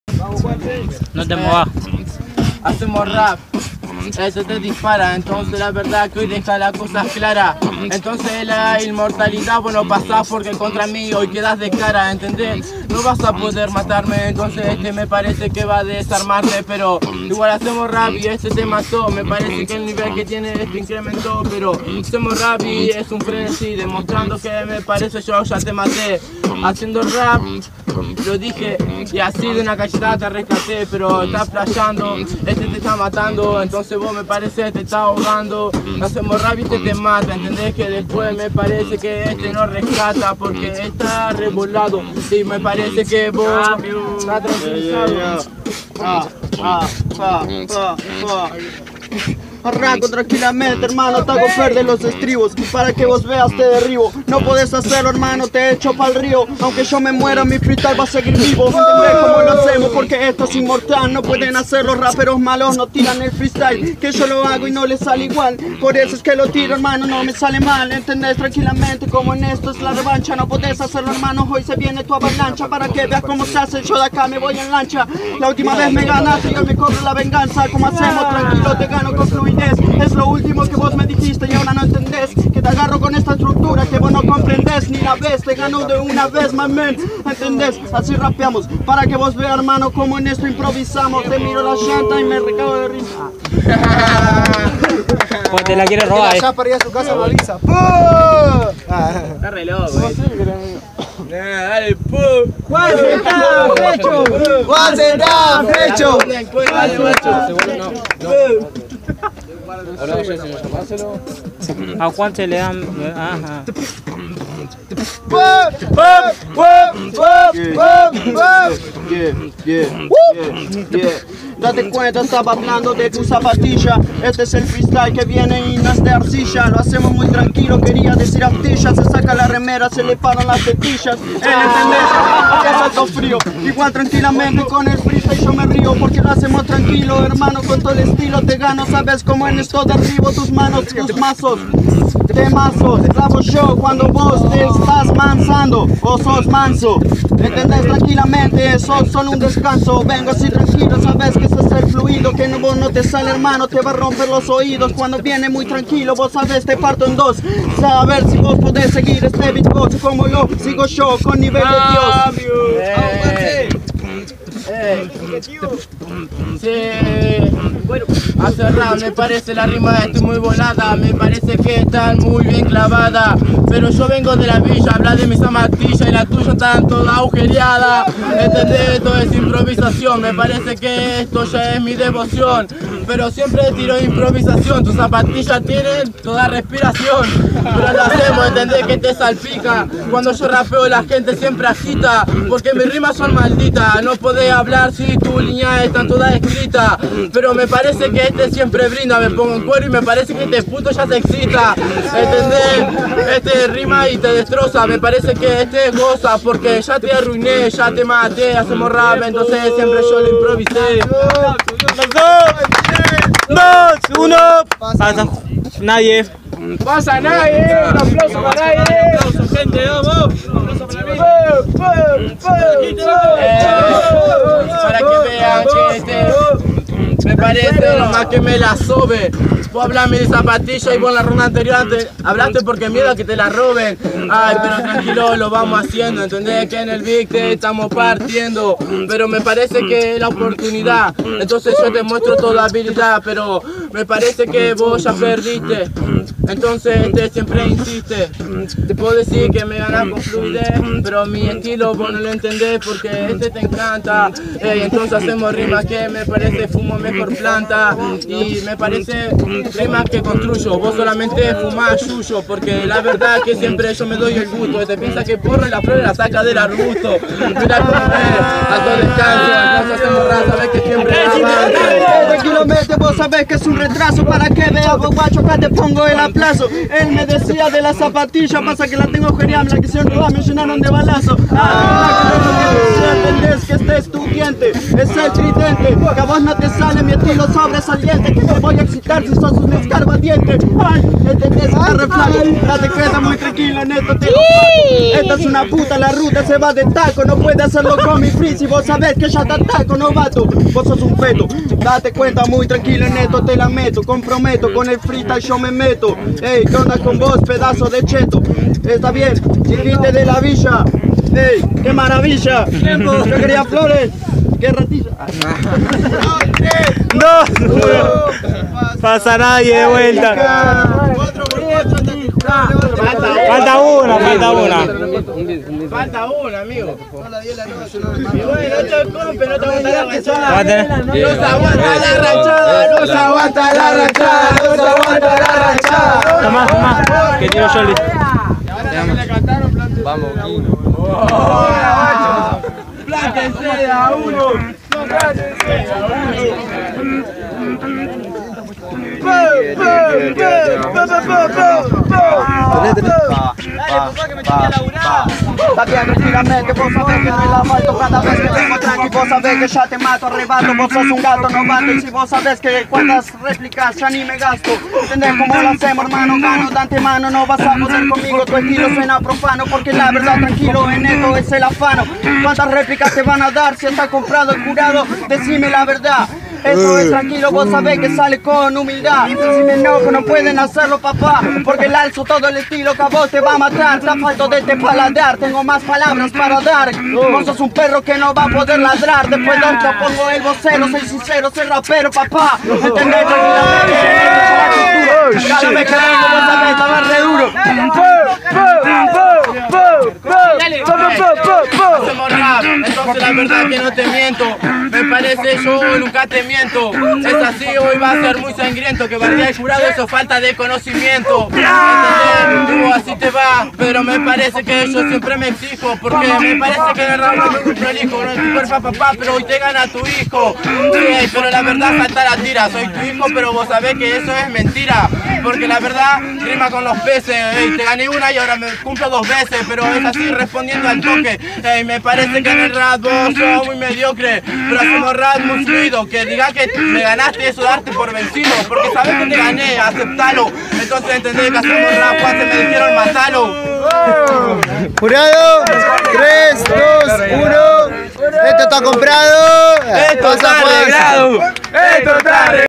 Juan-C vs Agus 8vo Rap Clique 13ma Edicion
Bueno , Salio la Trigésima edición de Rap Clique , se hace en la bajada del puente carretero (Santo Tomé)
Domingo 3 a las 19:00
1 vs 1 CON TEMÁTICA , Si hay replica es libre (SIN TEMÁTICA).
Compiten todos..

Falto: Venuz vs Santi paso Venuz

Jurado: Pupy - Gaby - Lea

Temática:  Inmortalidad

Ganador: Juan-C